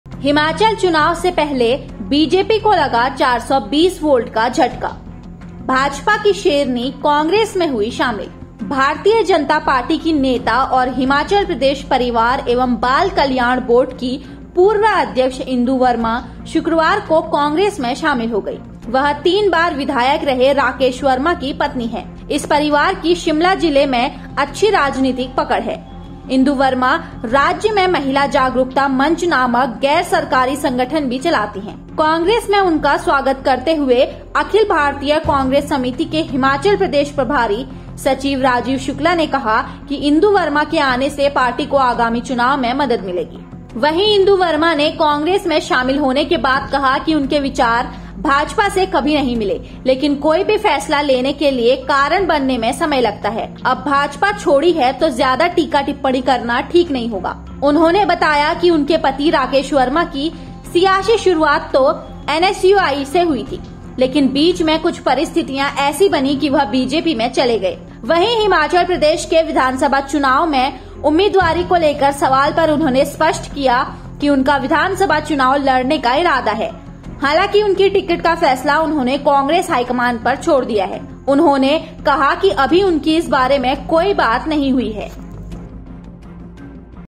हिमाचल चुनाव से पहले बीजेपी को लगा 420 वोल्ट का झटका, भाजपा की शेरनी कांग्रेस में हुई शामिल
हिमाचल चुनाव से पहले बीजेपी को लगा 420 वोल्ट का झटका, भाजपा की शेरनी कांग्रेस में हुई शामिल ||  | Congress | Priyanka | Rahul | Sonia | CWC meeting |